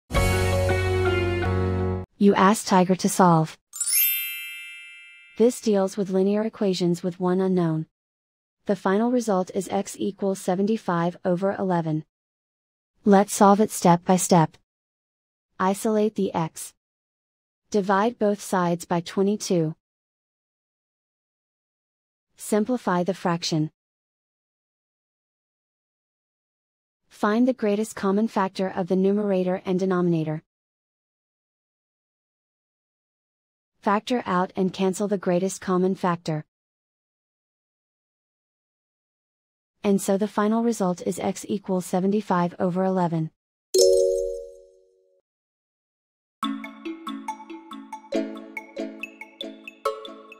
Linear equation with one unknown: Solve 22X=150 step-by-step solution
Solving 22X=150 using the linear equation with one unknown solver

for a step-by-step and more tips and tricks for dealing with linear equations with one unknown.